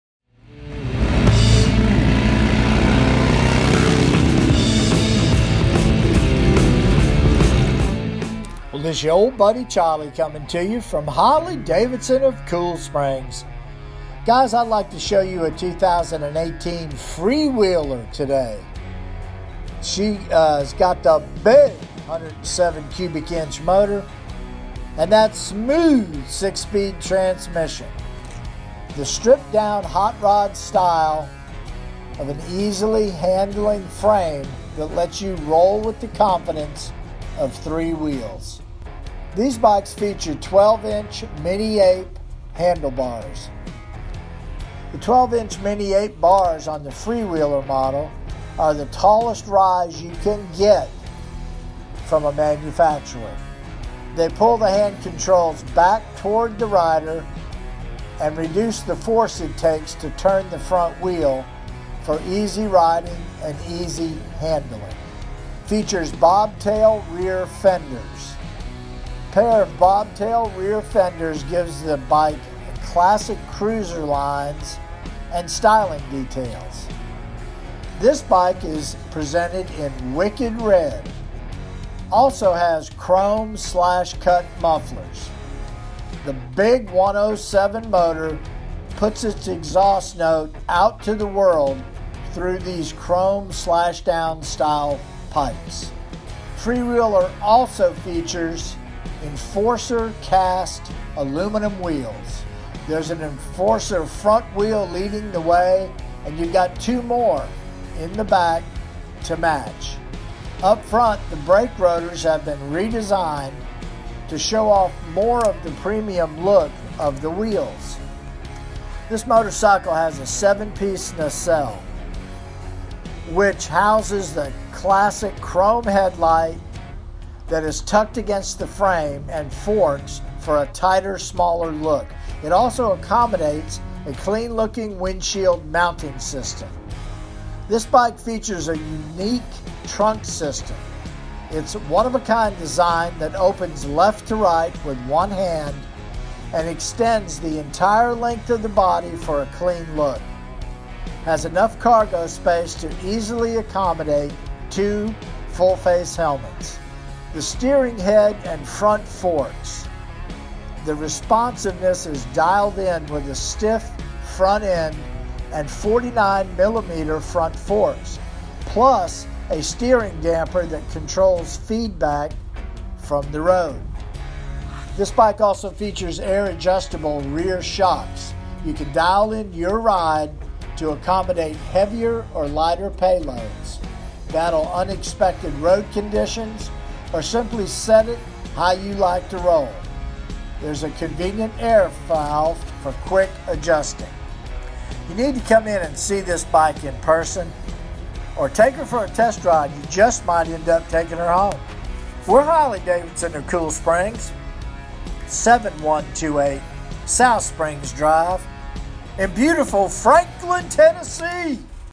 2018 Harley-Davidson® Freewheeler®
2018 Harley-Davidson® Freewheeler®
Stripped-down, hot rod style on an easy-handling frame that lets you roll with the confidence of three wheels.

Features may include:
12" Mini-Ape Hanger Bars
The 12-inch Mini-Ape hanger bars on the Freewheeler® model are the tallest rise you can get from an OE manufacturer. They pull the hand controls back toward the rider, and reduce the force it takes to turn the front wheel for easy riding and easy handling.

Bobtail Rear Fenders
The pair of Harley-Davidson® bobtail rear fenders gives the bike classic cruiser lines and styling details.

Chrome Slash Cut Mufflers
The Milwaukee-Eight® 107 engine puts its exhaust note out to the world through these chrome slash down style pipes.

Enforcer Cast Aluminum Wheels
There’s an Enforcer front wheel leading the way, and you've got two more to match in the back. Up front, the brake rotors have been re-designed to show off more of the premium custom look of the wheels.

7-Piece Nacelle
A classic style chrome headlight nacelle that is tucked tight against the frame and forks for a tighter, smaller look. It also accommodates a clean-looking windshield mounting system.

Unique Trunk System
It's a one-of-a-kind design that opens left to right (with one hand) and extends the entire length of the body for a clean look. Enough cargo space to easily accommodate two full-face helmets.

Milwaukee-Eight® V-Twin Engine
A powerful, smooth-running engine with crisp throttle response and a pure, soul-satisfying rumble.

Steering Head and Front Forks
The responsiveness is dialed in with a stiff front end and 49mm front forks, plus a steering damper that controls feedback from the road.

Air Adjustable Rear Shocks
You can dial in your ride to accommodate heavier or lighter payloads, battle unexpected road conditions, or simply set it to how you like to roll. There's a convenient air valve for quick adjusting.